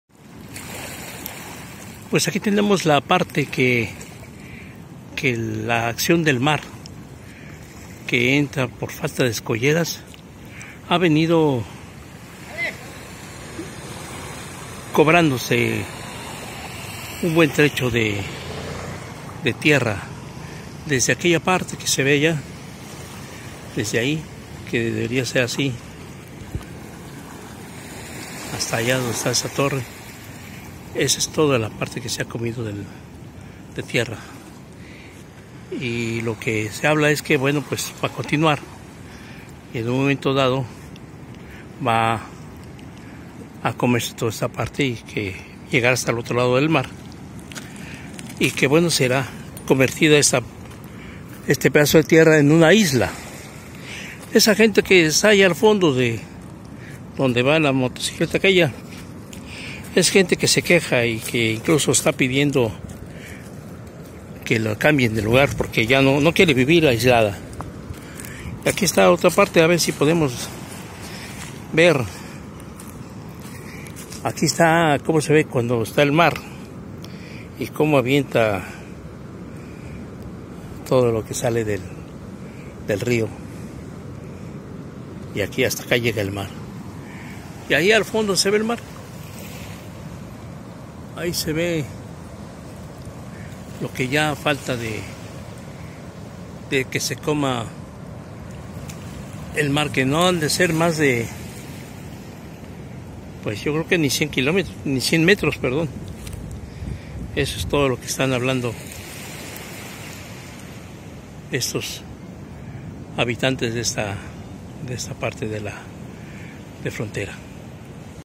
...el daño a poblados por la falta de escolleras, es palpable
...casi la totalidad de los 25,267 km² de superficie del estado de Tabasco es una llanura inundable formada por las lluvias y los ríos desbordados la cual durante siglos ha convivido con inundaciones por lo que no se explica cómo los gobiernos y la población no han sabido lidiar con ellas. Villahermosa, la capital, que cuenta con una altura promedio de 9 metros sobre el nivel del mar (msnm), por tal motivo, las escolleras permitirán el paso libre del caudal del Grijalva desazolvando su desembocadura...

Esto y toda la información sobre la falta de escolleras en el Grijalva,